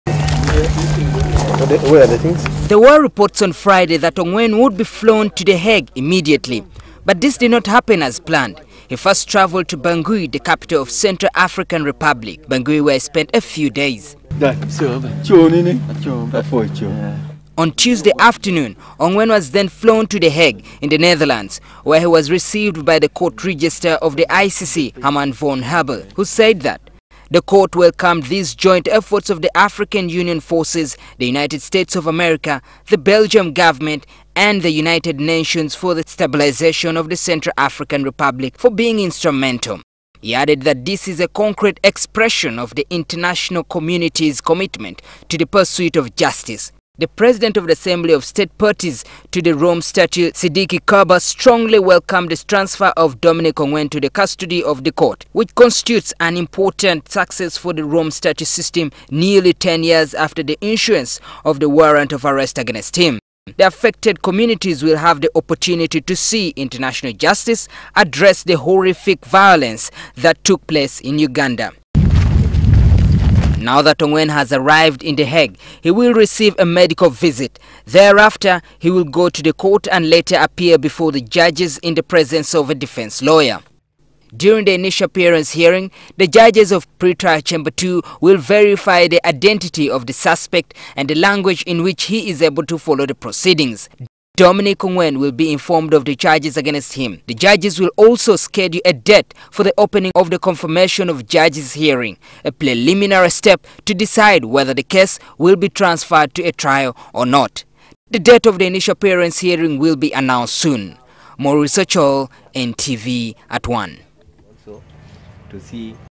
Dominic Ongwen arrives at the ICC
Former LRA Third- In- Command Dominic Ongwen has finally arrived in the Hague in the Netherlands after his capture in the Central African Republic about two weeks ago. Ongwen is among the 5 rebel warlords the International Criminal Court issued arrest warrants for. They are accused of committing crimes against humanity and war crimes. Ongwen was received by the Registrar of the ICC Herman von Hebel and will be in the custody of the Court, nearly ten years after the issuance of the warrant of arrest was issued against him.